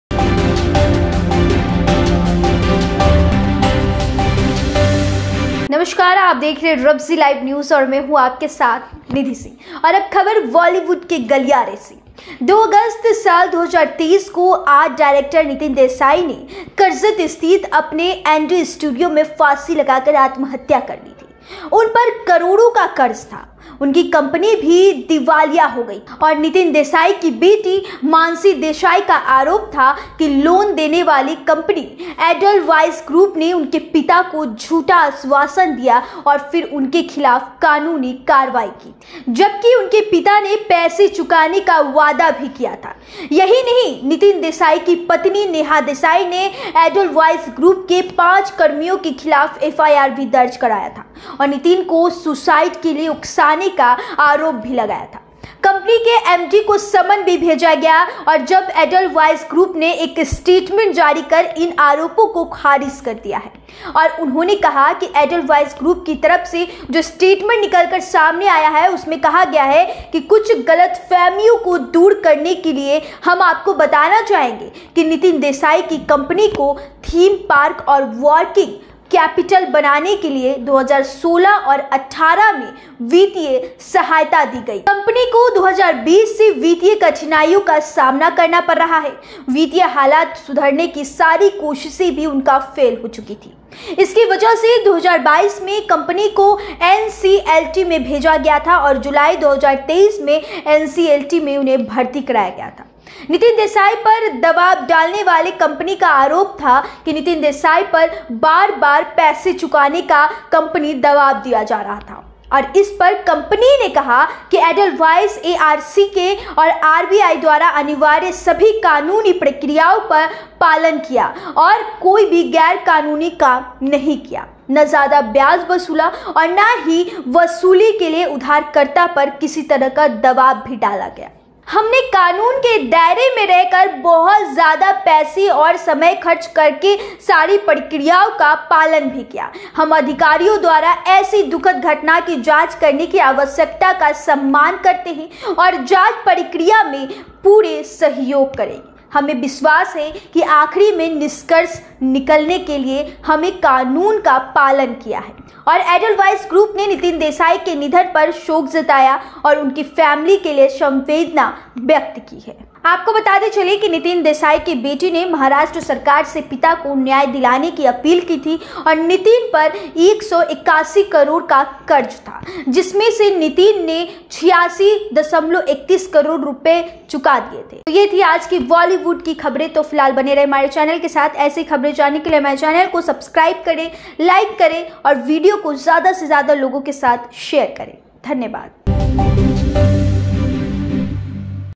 नितिन देसाई केस में आरोप में फाइनेंस कंपनी नें जारी किया अपना स्टेटमेंट#nitindesaisuicide
नितिन देसाई केस में आरोप में फाइनेंस कंपनी नें जारी किया अपना स्टेटमेंट